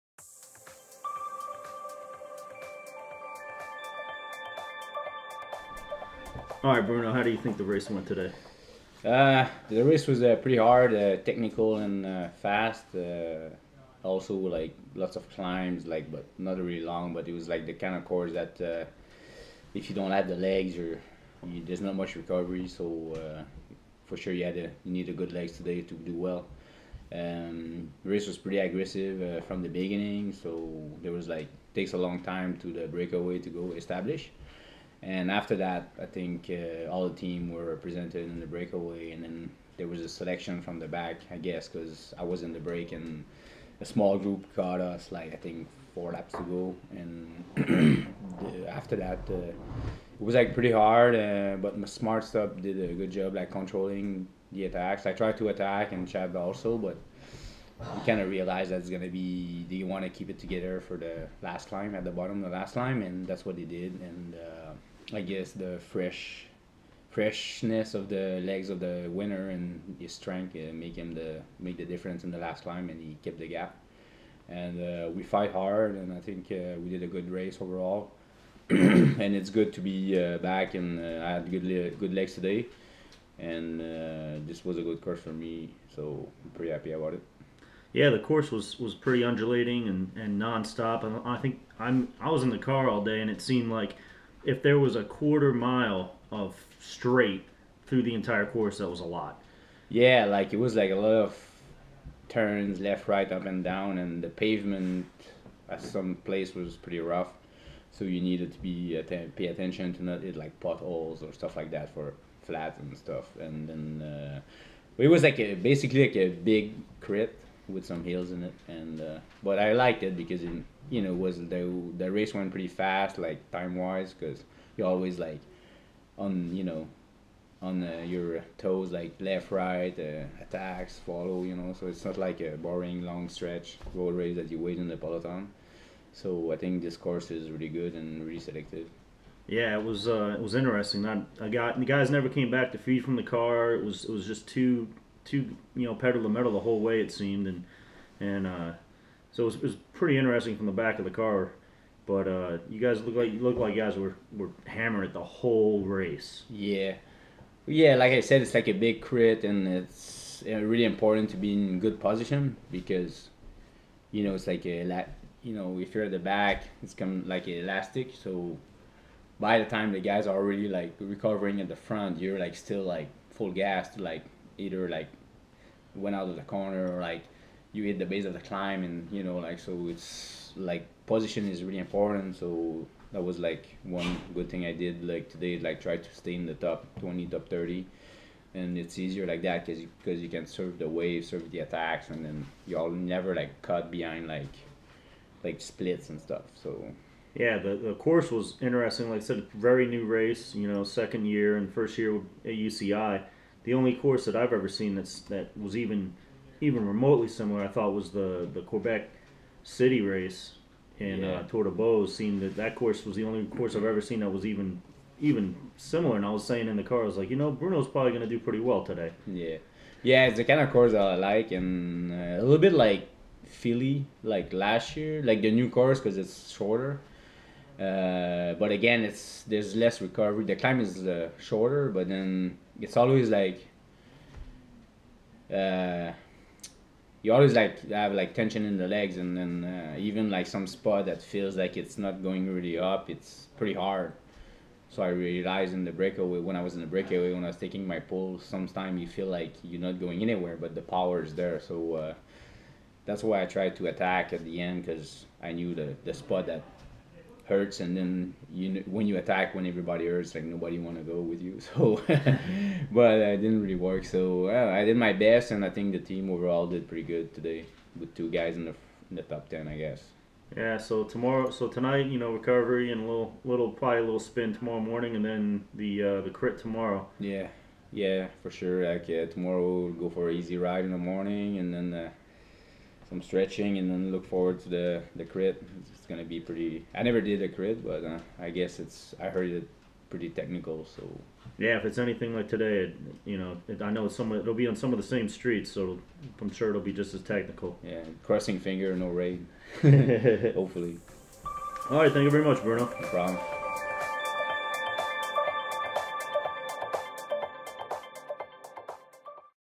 Winston Salem Cycling Classic UCI 1.2 Road Race 2014
Bruno Langlois recaps the Winston Salem Cycling Classic UCI 1.2 Road Race 2014